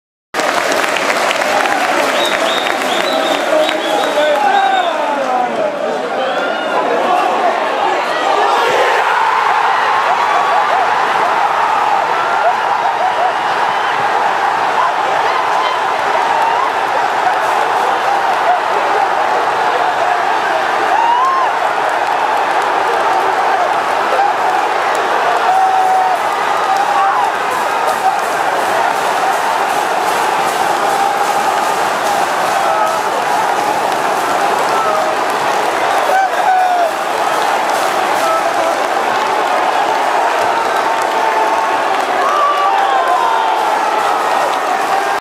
Shaun Miller scores the last minute winner at Fulham
Shaun Miller's goal from our position on the Putney End